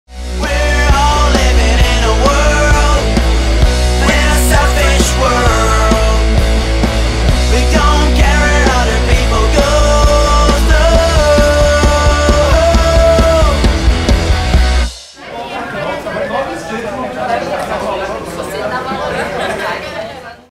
Teaser: Pocket Show The Sinks + Kung Fu Johnny.
Sexta, dia 15 de março assista pocket show do The Sinks e Kung Fu Johnny.